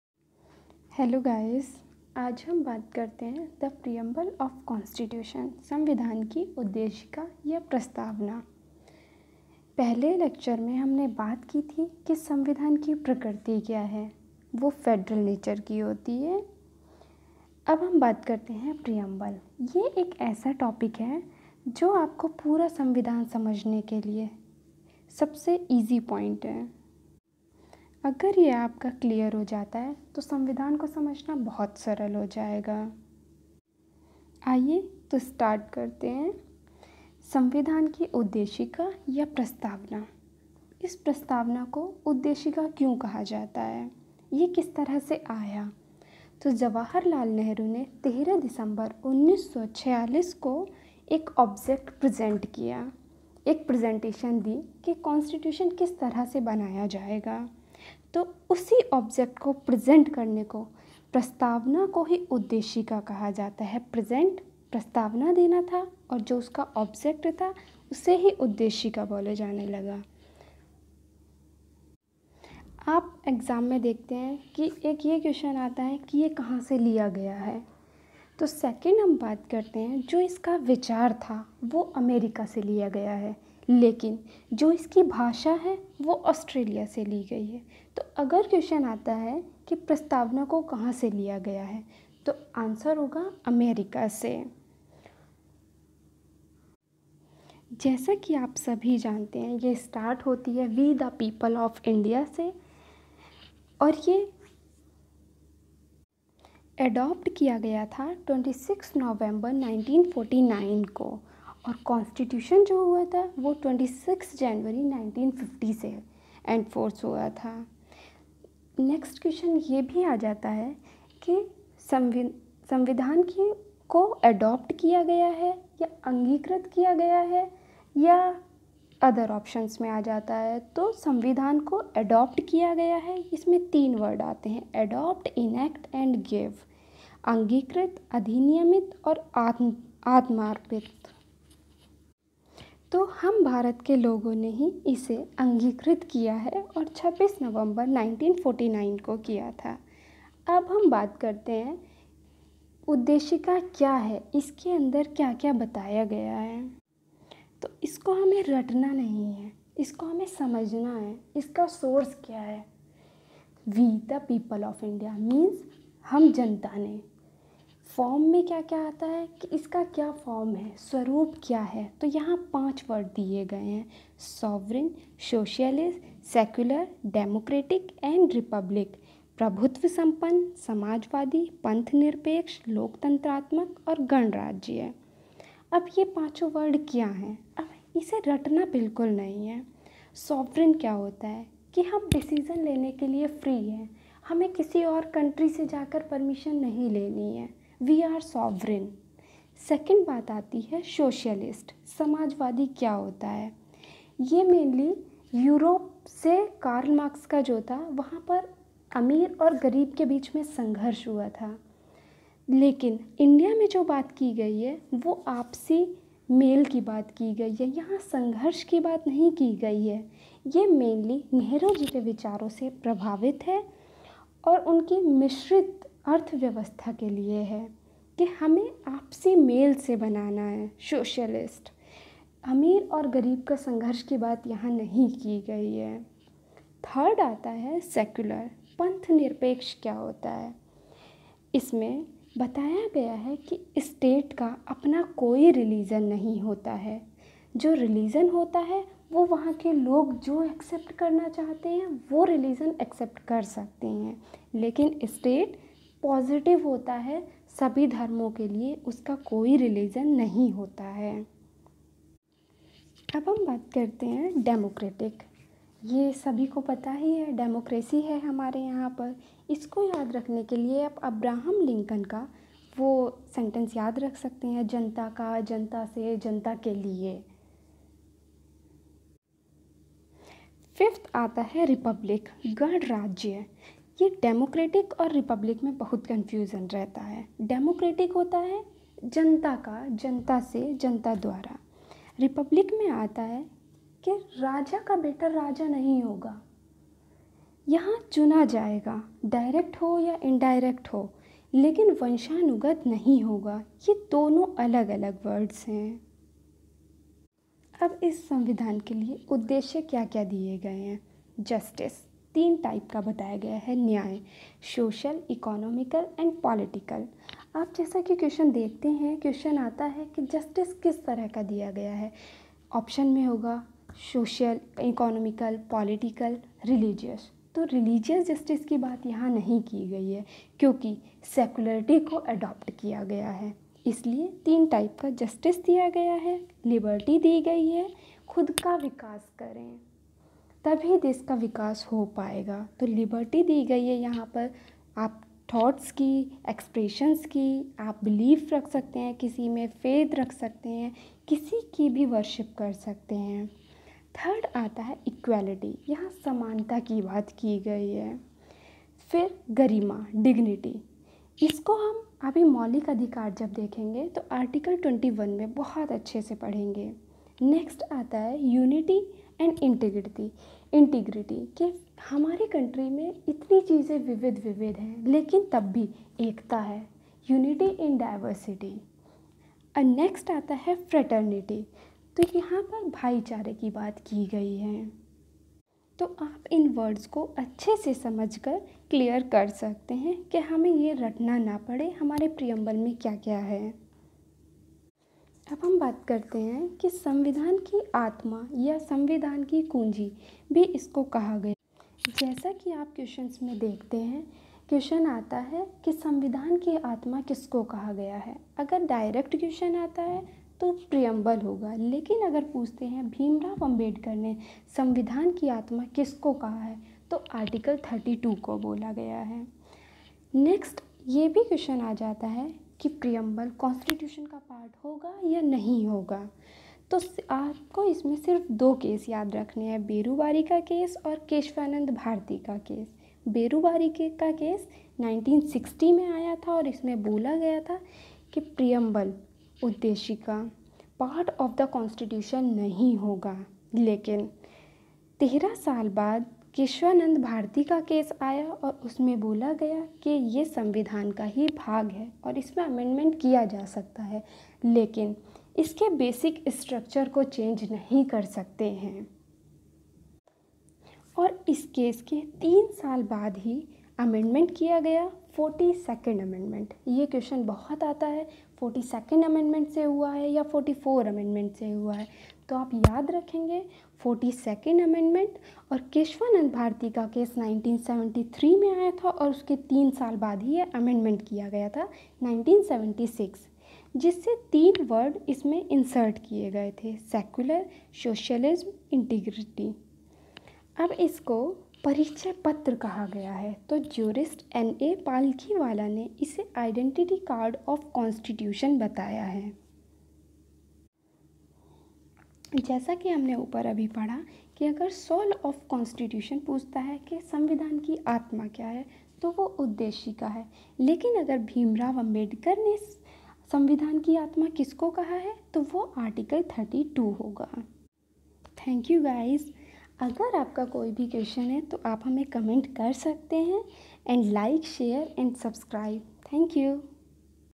The constitution of India #2
Preamble of Indian Constitution
"We the people of India. "
Beneficial for pcsj
The key of the Constitution